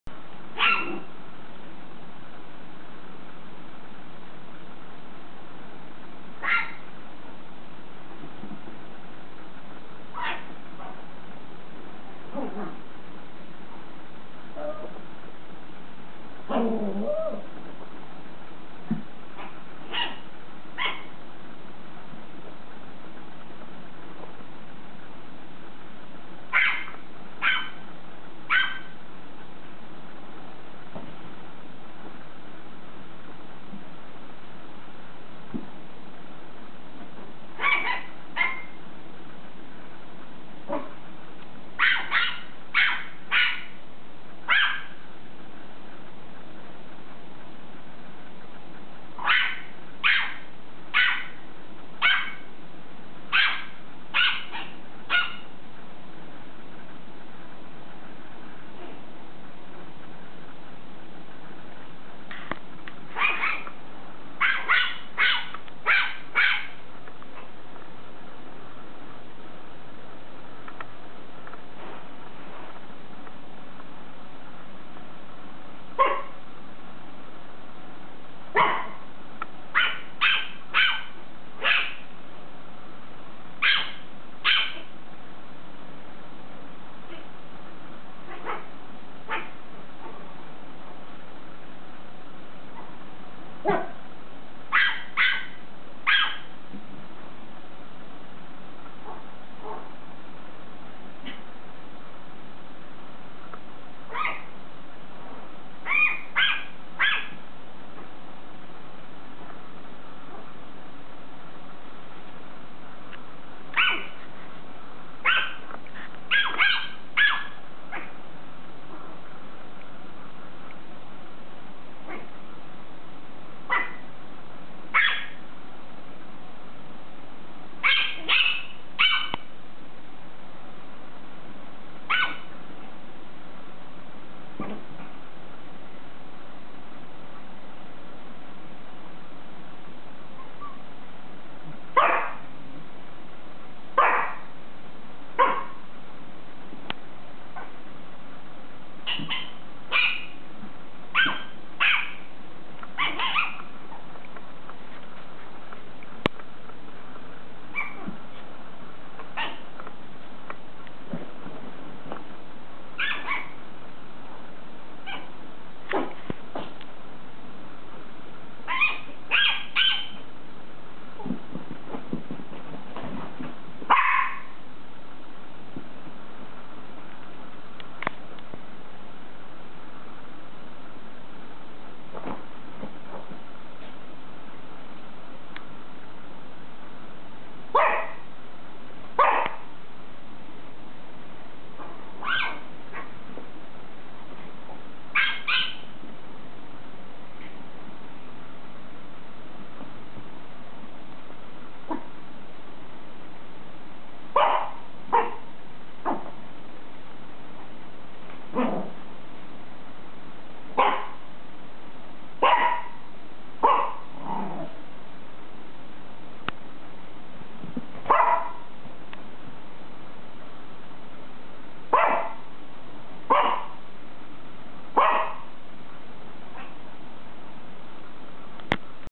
Paris the Maltese puppy tormenting the other dogs
Diva Dog Paris torments Zoe, Bella, Oliver and Gabby from under the furniture. No that is funny.
Posted by Barking Divas Chic Pet Boutique, 7238 Thomas Dr Suite P, Panama City Beach FL.